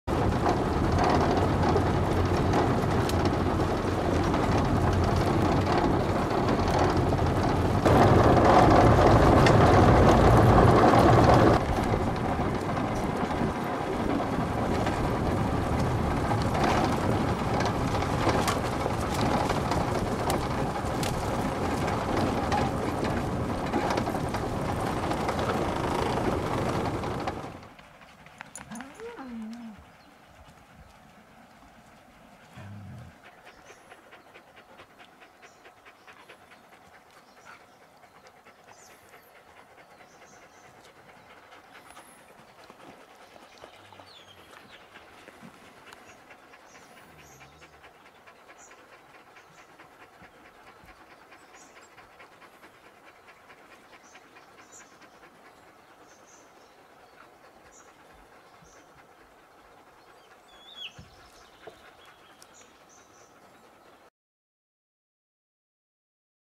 Summer training with sleddogs at Laplandhusky in north Sweden.
Slädhundar i försäsongsträning en sommarnatt i norra Sverige.
Sled dogs in pre-season training a summer night in northern Sweden.
You can see more photos and the tours we offer or on Facebook and Instagram, you will found it if you searching onLaplandhusky.